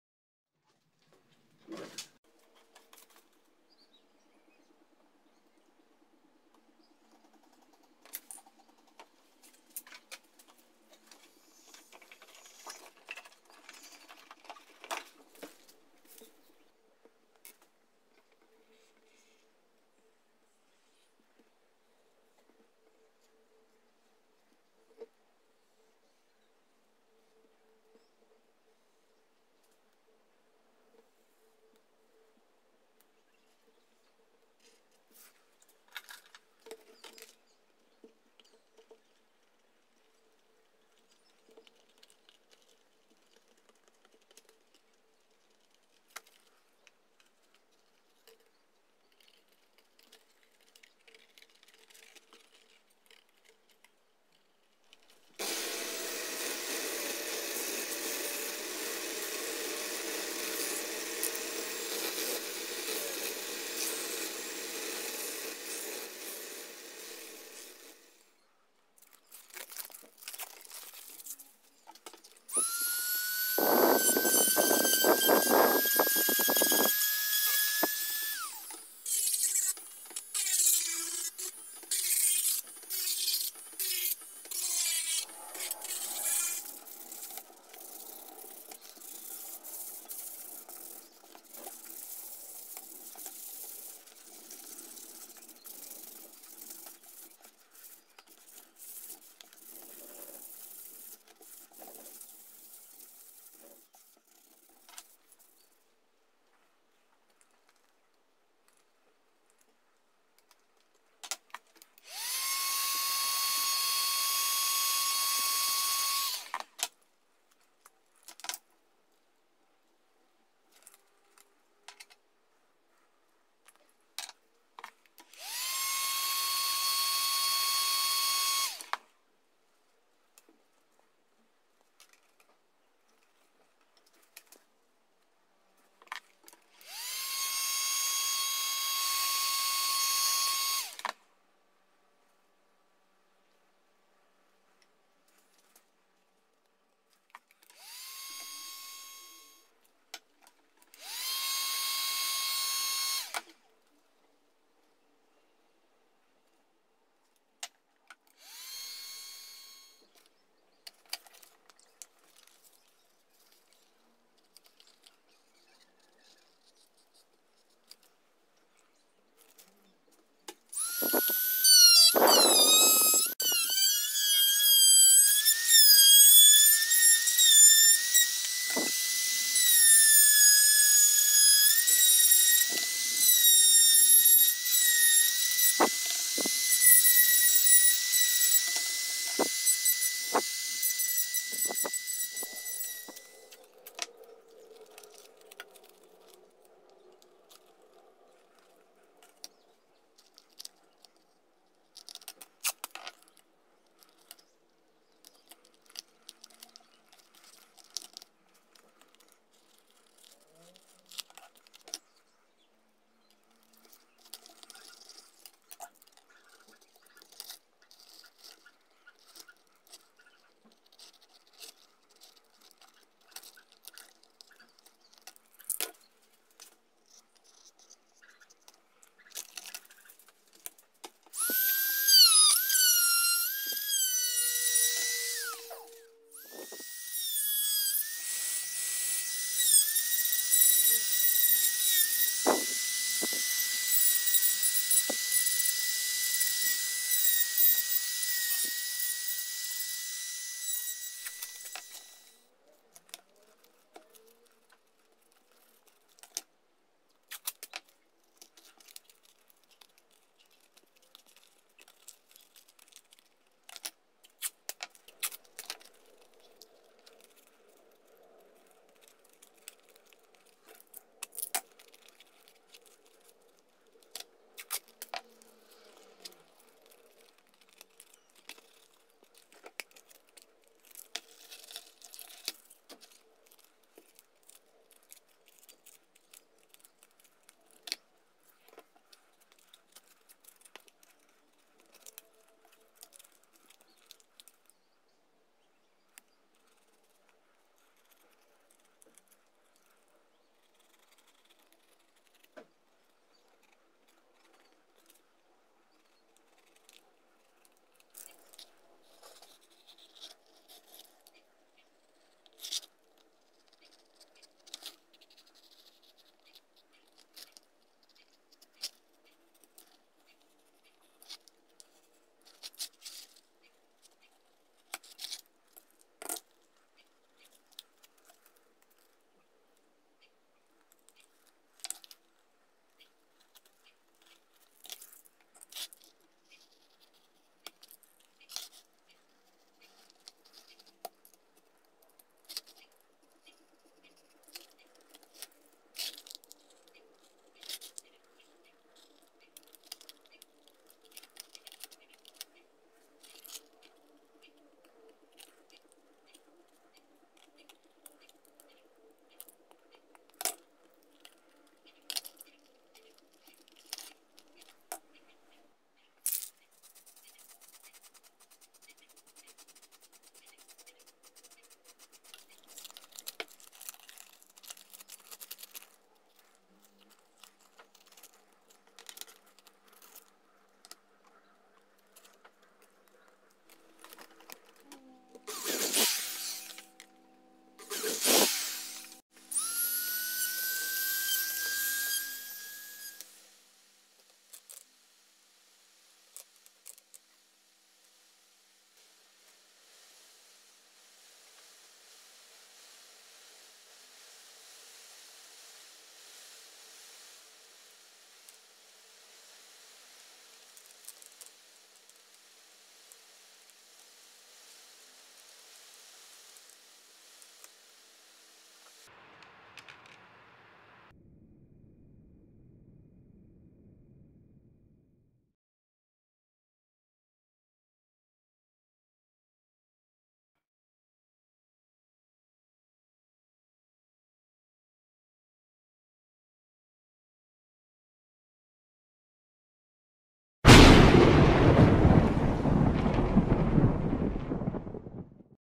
Making Carnival Rabbit Mask - Bioshock
I made this mask from 1mm PVC sheet. It's heat-formed and spray painted. Quick weekend project before masquerade party. Mask is designed for specific persons face so I won't be sharing templates this time.